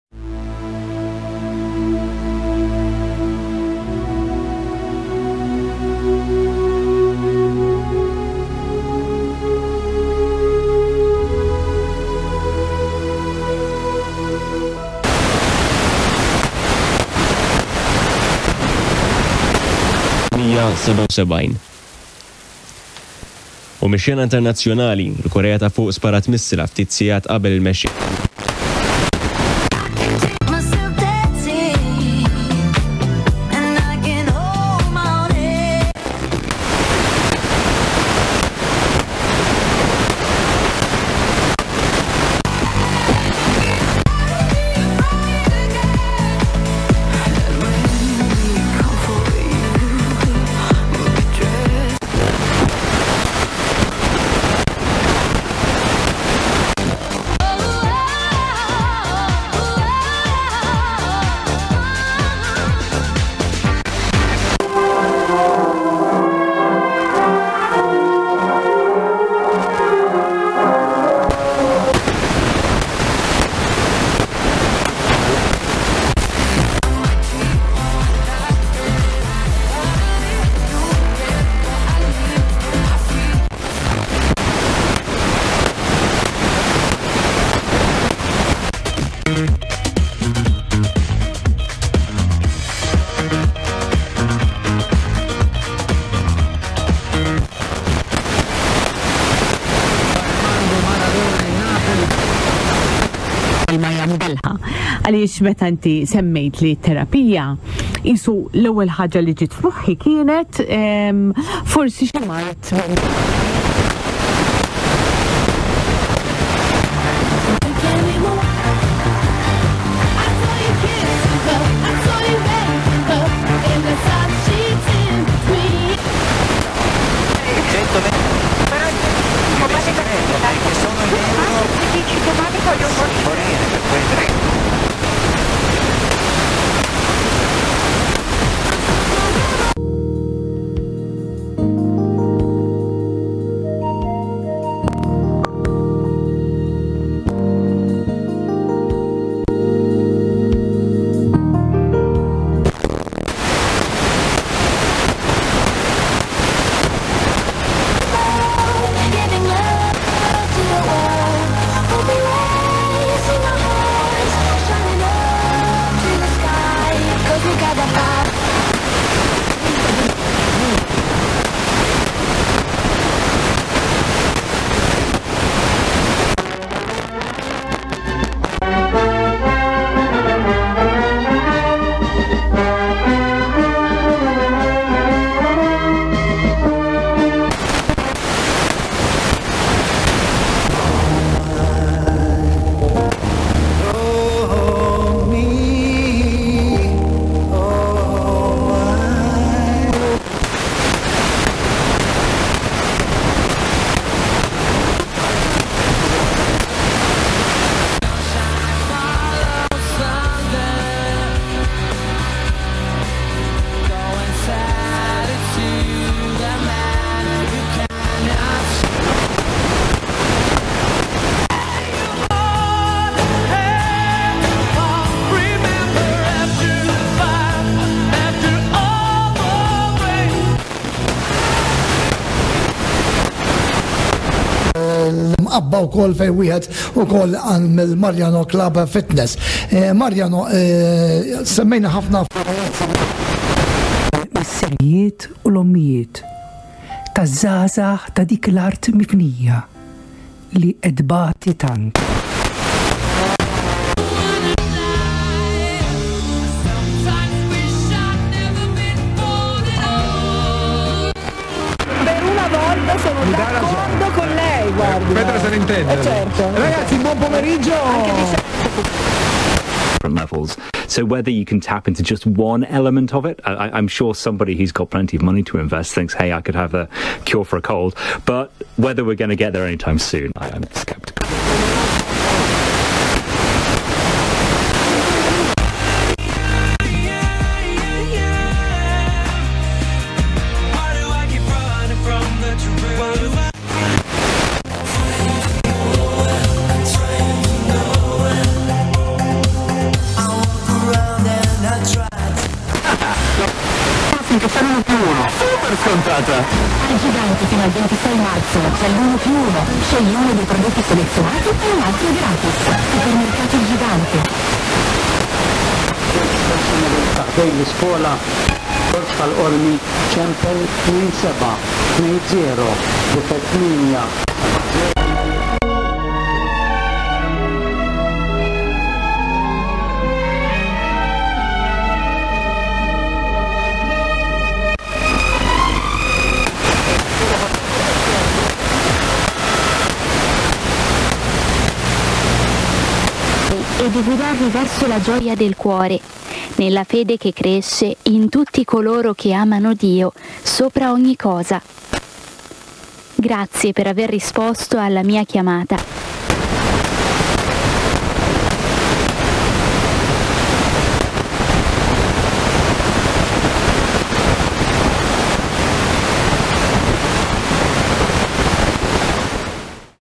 Valletta, Malta FM Bandscan (March 2023)
Amasondx goes international! (Again) This bandscan was taken in Valletta, Malta on March 16, 2023. Malta's a small enough country that most stations don't put out more than a few kW, and you can hear almost every station in the country from most parts of the island.